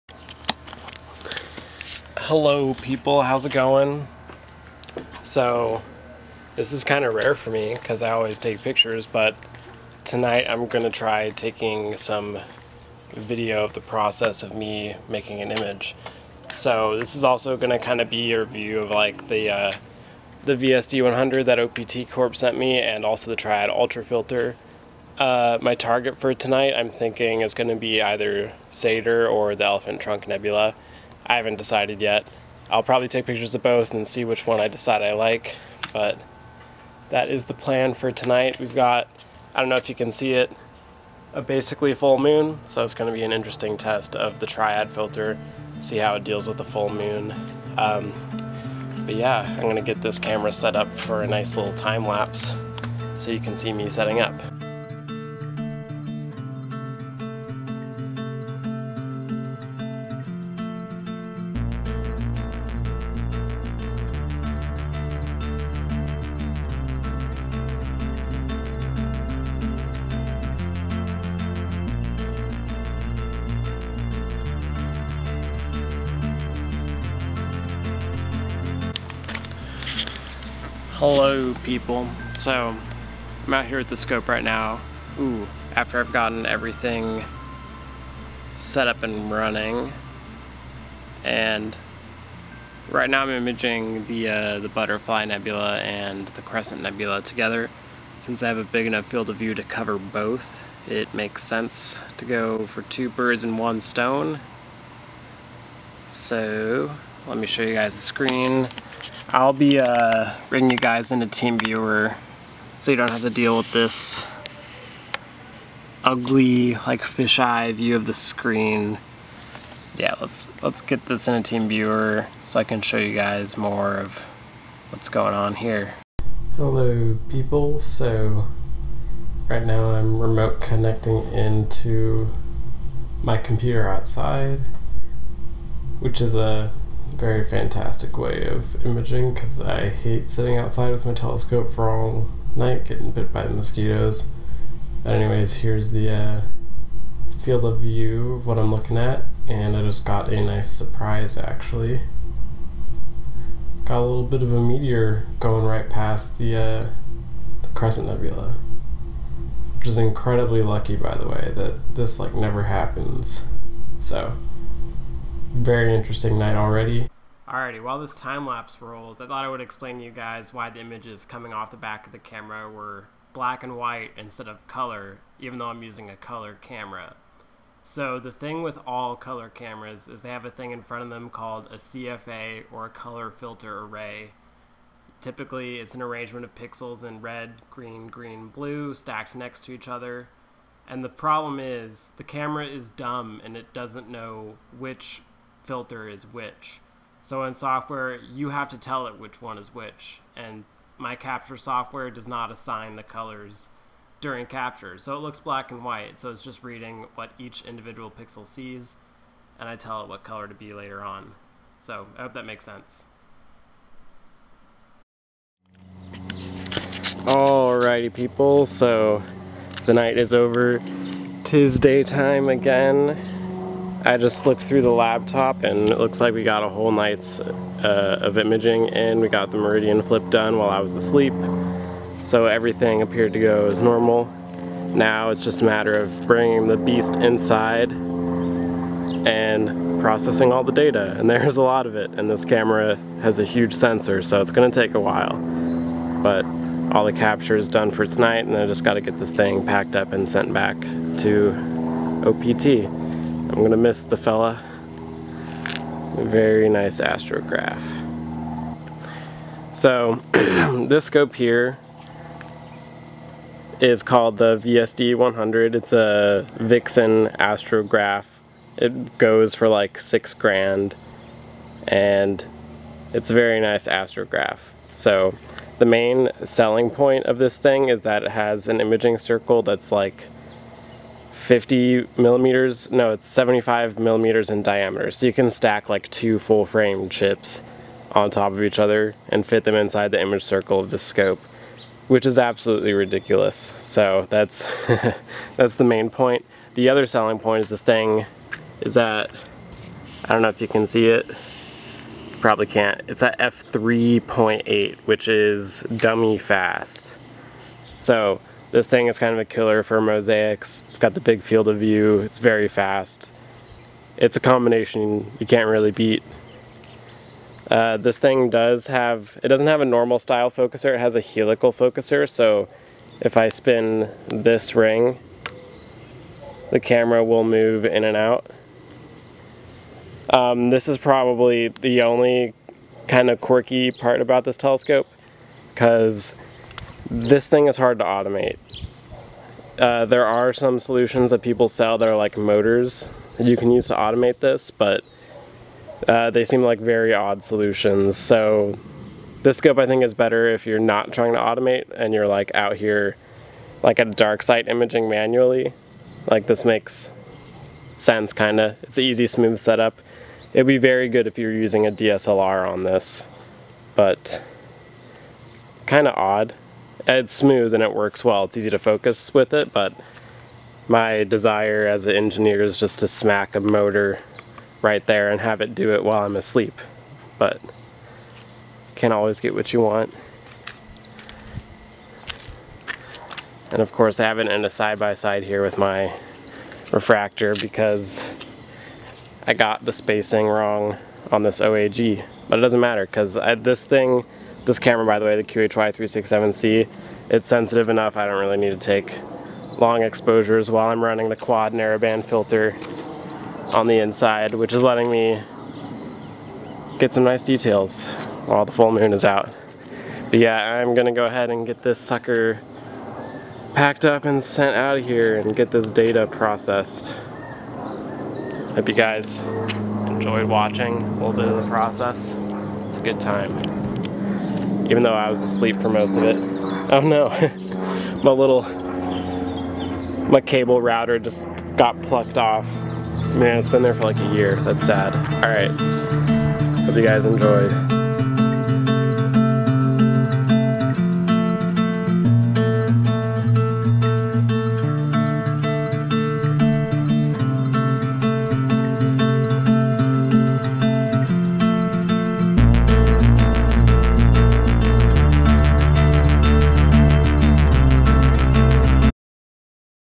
Capturing Cygnus with the Vixen VSD100 f/3.8
Hey everyone here's a little video I made going along the capture process in making an astro image. I hope you guys enjoy.

If you want to pick up a QHY367c or VSD100 or triad ultra filter, please check out these links: